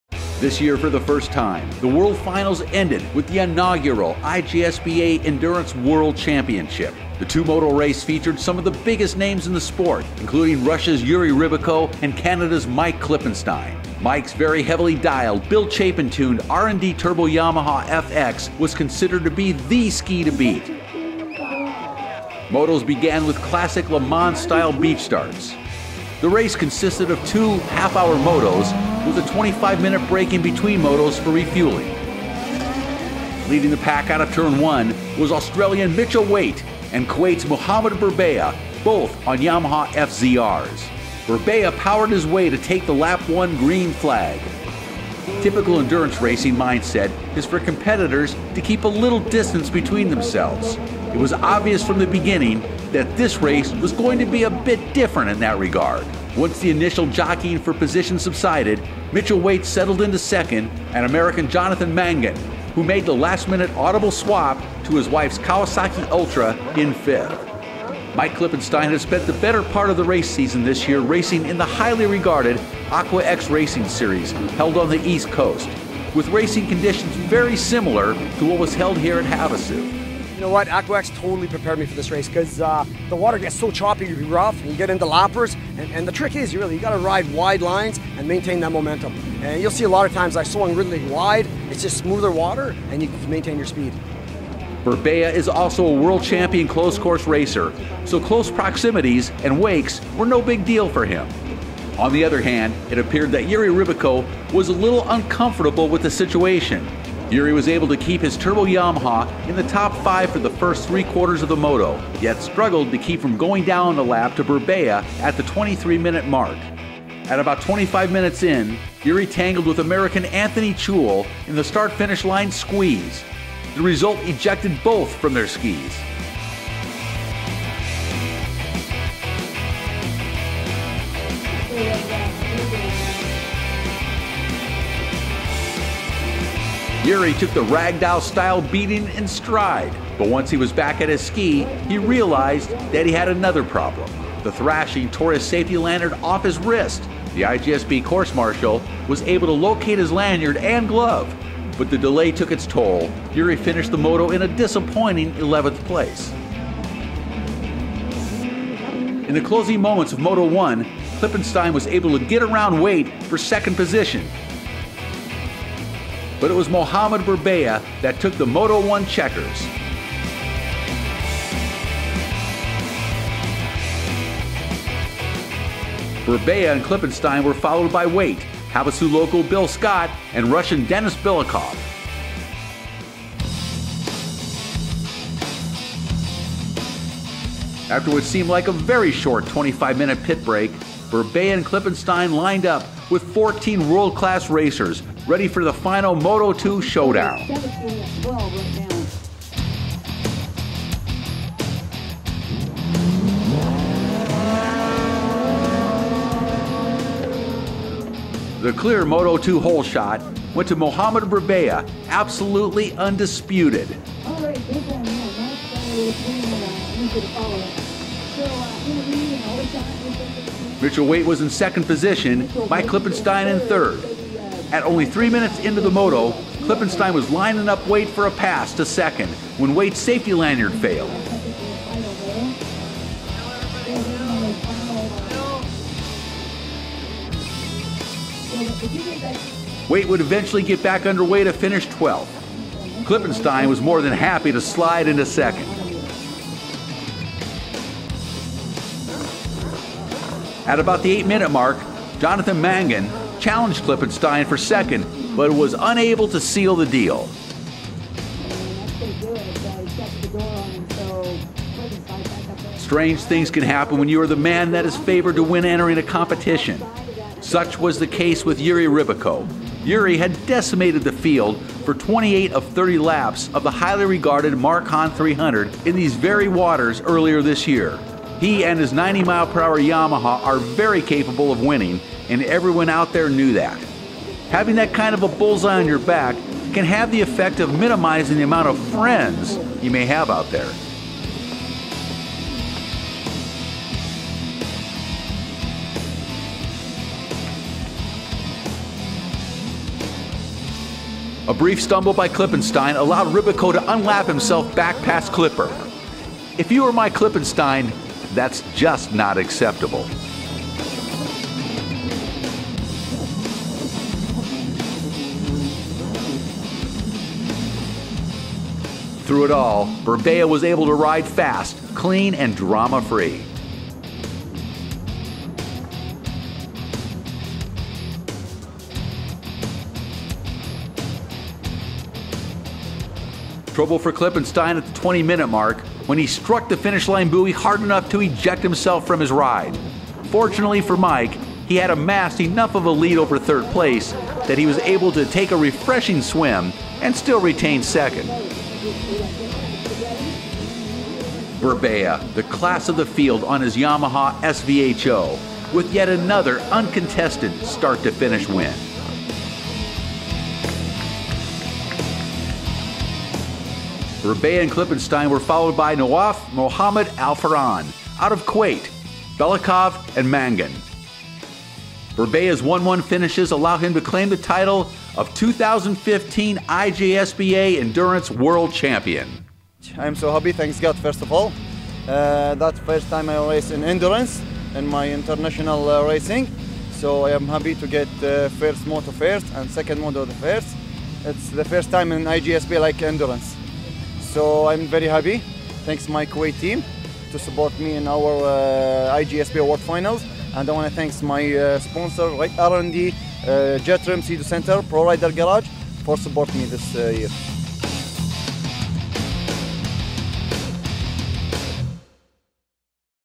2015 IJSBA World Finals Endurance
Endurance racing is generally considered a long drawn-out affair with little side-by-side racing. The inaugural IJSBA World Finals Endurance Championship proved otherwise...in a very big way featuring as much bar banging, fiberglass bashing action, if not more, than most closed course races. Emerging from the mayhem with the title was Kuwait's Mohammed Burbayea on his Yamaha SVHO.